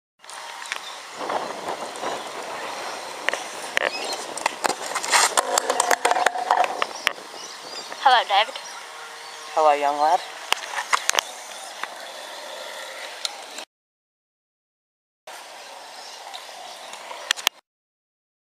whip bar flat
this is one of my first whip to barspins flat. pretty stoked.
filmed by my brother.